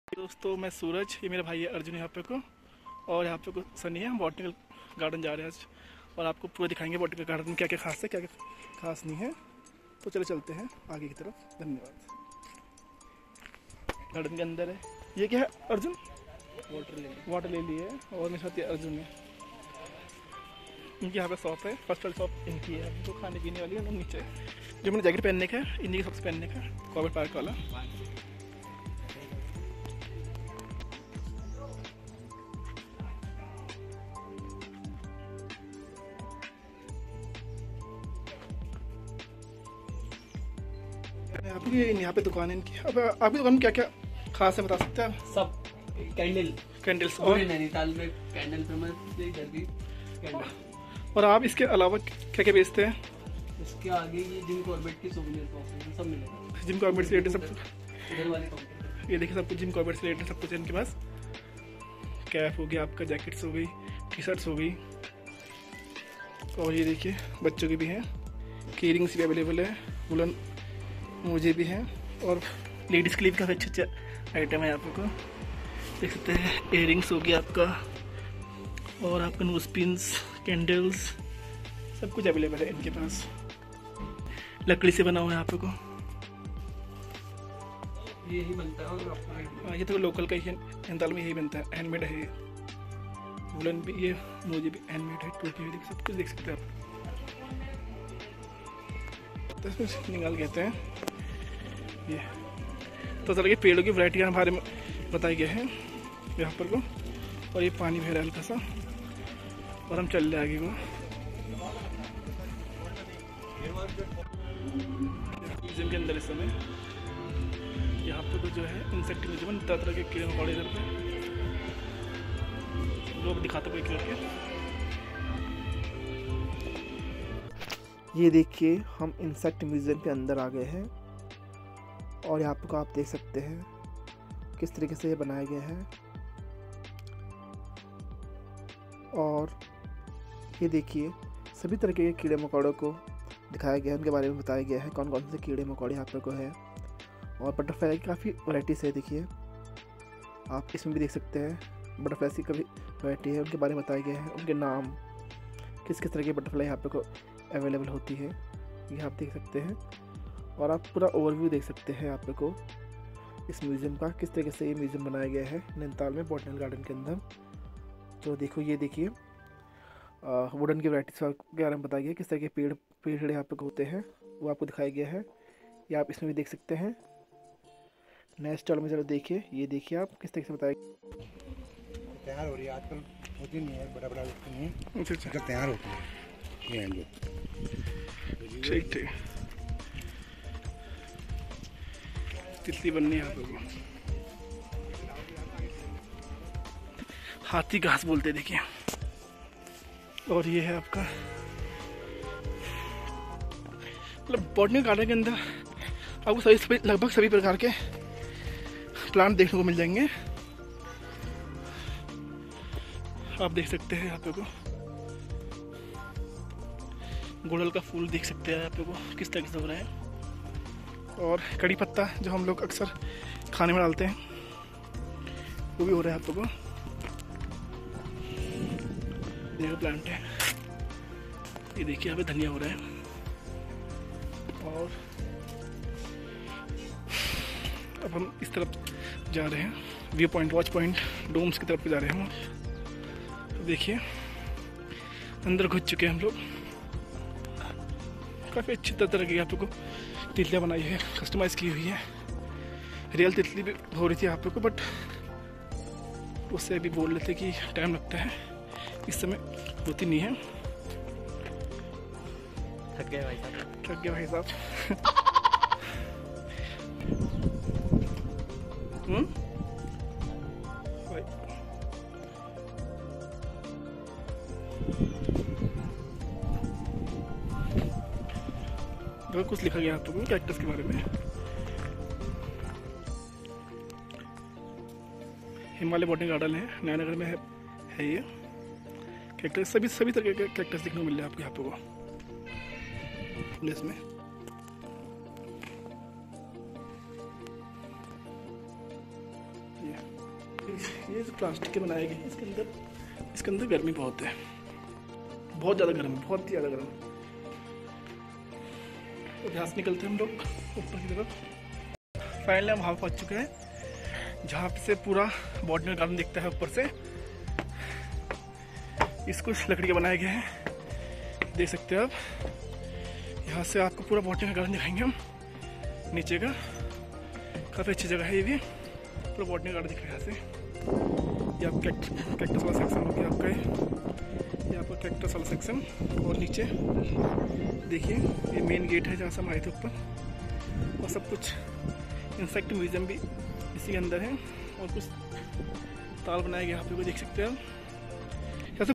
उत्तराखण्ड में एक रहस्यमय जगह 😱 || Botanical Garden nainital
उत्तराखण्ड में एक रहस्यमय जगह 😱 || botanical Garden nainital || nainital mystery

Hello dosto uttarakhand ke nainital main botanical Garden hain.. jaha aap aa kar bhut kuch jaan skte hain plant's ke baare main. ek baar visit jarur kare bhut hi achi jagah hain!!